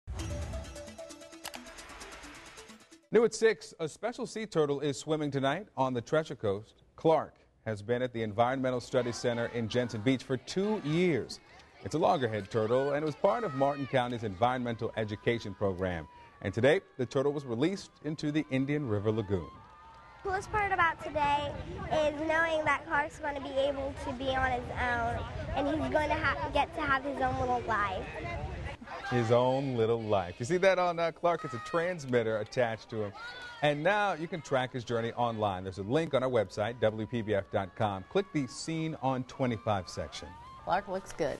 Clark Returns To The Water
Clark, Martin County's favorite loggerhead turtle, was released into the Indian River Lagoon after spending the last two years at the Environmental Studies Center in Jensen Beach.